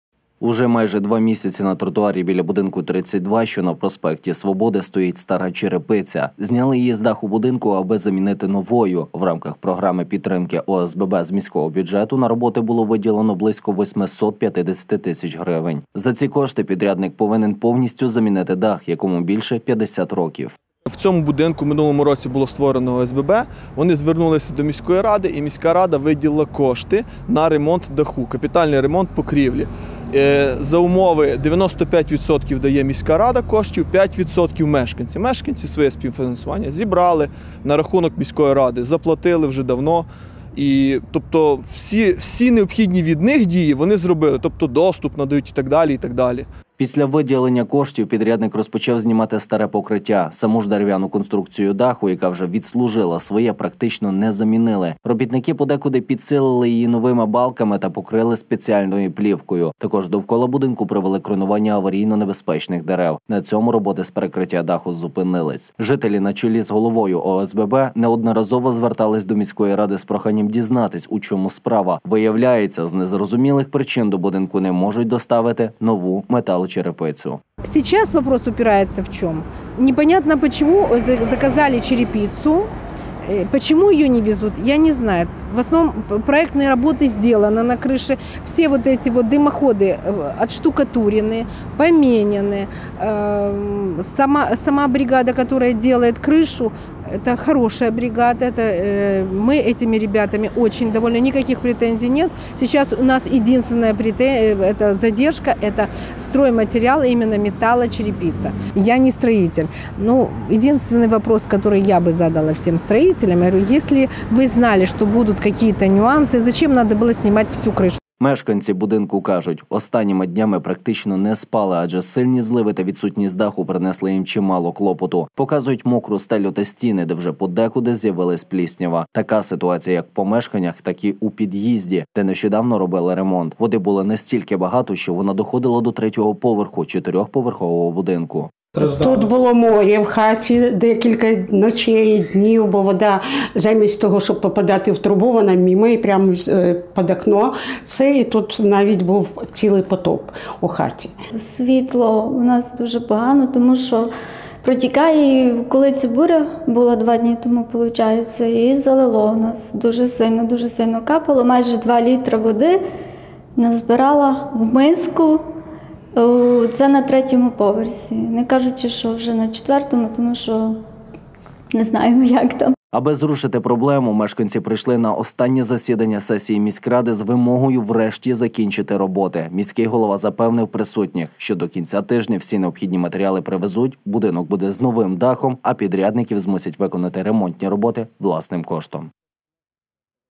Багатоповерхівку без перекриття в Ужгороді залило
Уже майже два місяці на тротуарі біля будинку 32, що на Проспекті Свободи, стоїть стара черепиця. Зняли її з даху будинку, аби замінити на нову. В рамках програми підтримки ОСББ з міського бюджету на роботи було виділено близько восьмисот п’ятдесяти тис. грн. За ці кошти підрядник повинен повністю замінити дах, якому більше 50 років.
Після виділення коштів, підрядник розпочав знімати старе покриття. Саму ж дерев’яну конструкцію даху, яка вже відслужила своє, практично не замінили. Робітники подекуди підсилили її новими балками та покрили спеціальною плівкою. Також довкола будинку провели кронування аварійно небезпечних дерев. На цьому роботи з перекриття даху зупинились. Жителі, на чолі з головою ОСББ неодноразово звертались до міської ради з проханням дізнатись, у чому справа. Виявляється, з незрозумілих причин до будинку не можуть доставити нову метало-черепицю.
Мешканці будинку кажуть, останніми днями практично не спали, адже сильні зливи та відсутність даху, принесли їм чимало клопоту. Показують мокру стелю та стіни, де вже подекуди з’явилась пліснява. Така ситуація як в помешканнях так і у під’їзді, де нещодавно робили ремонт. Води було настільки багато, що вона доходила до третього поверху чотирьохповерхового будинку.
Аби зрушити проблему мешканці прийшли на останнє засідання сесії міськради з вимогою врешті закінчити роботи. Міський голова запевнив присутніх, що до кінця тижня всі необхідні матеріали привезуть, будинок буде з новим дахом, а підрядників змусить виконати ремонтні роботи власним коштом.